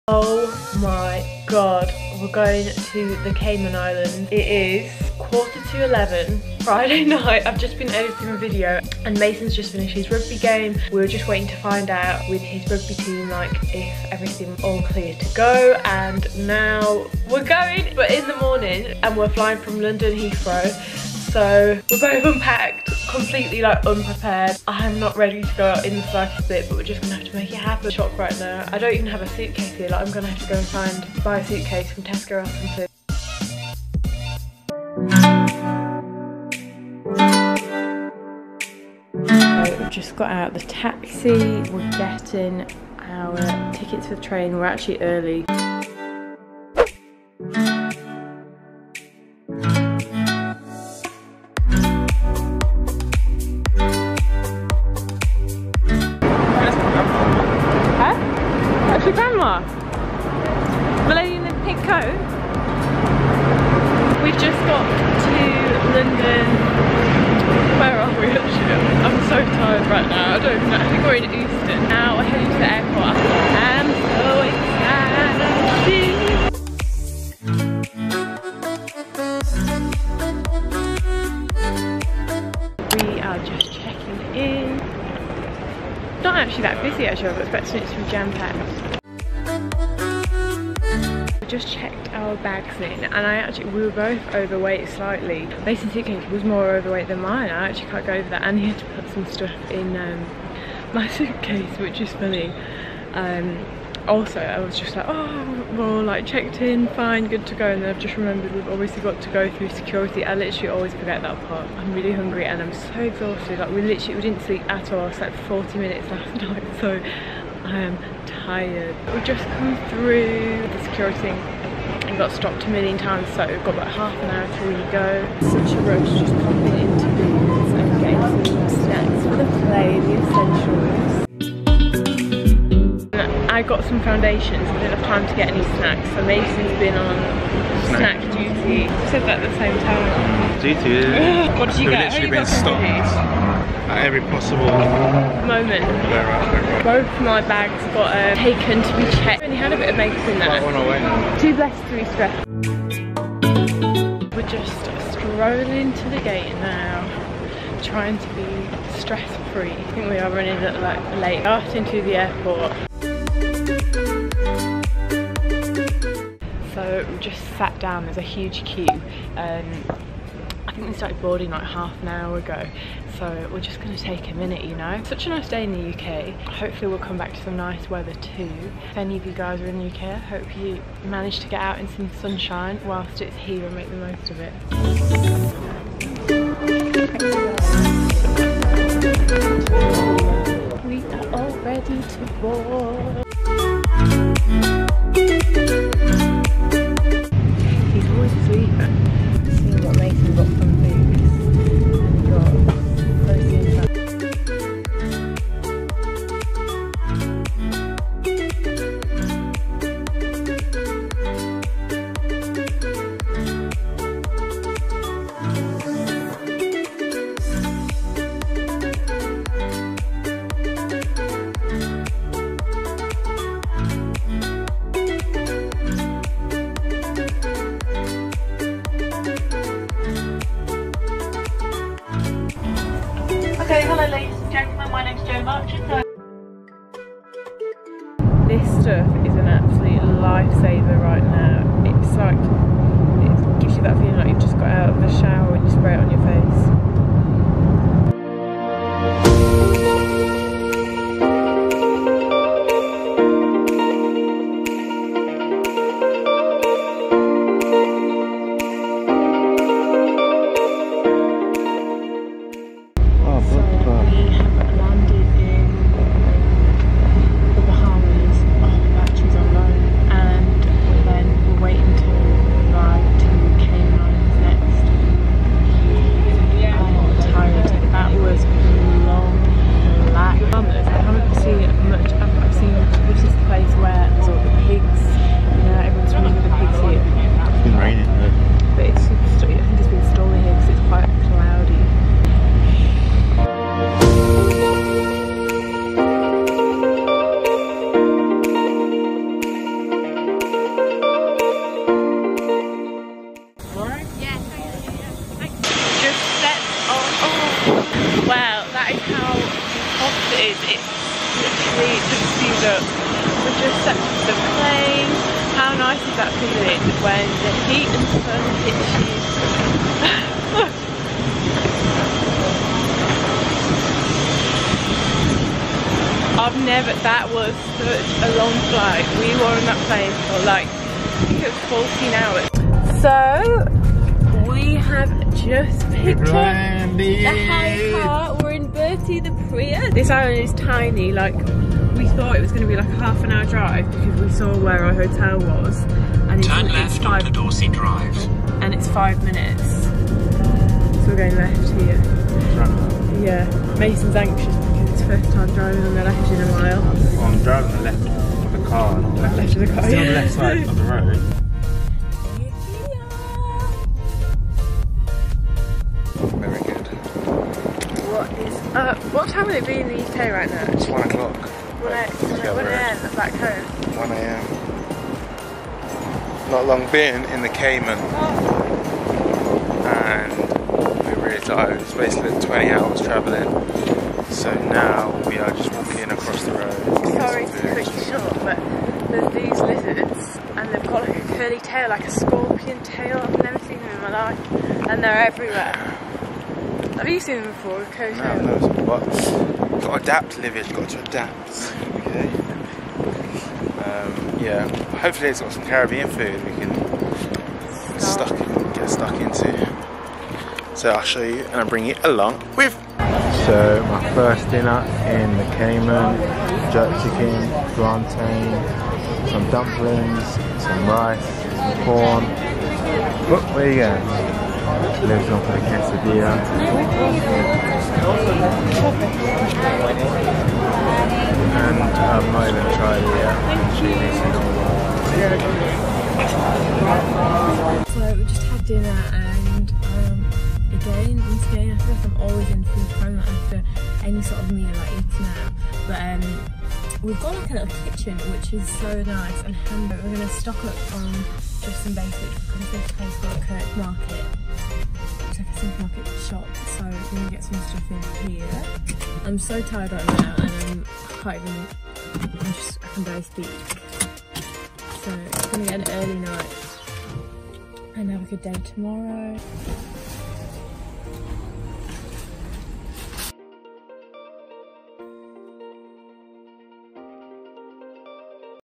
COME TO THE AIRPORT WITH ME / TRAVELLING TO CAYMAN ISLAND
HELLOO my loves!! Really hope you enjoy this lil fun vid travelling to the Cayman Islands 2019 with Mason and I!

Come say hey on Instagram! ♡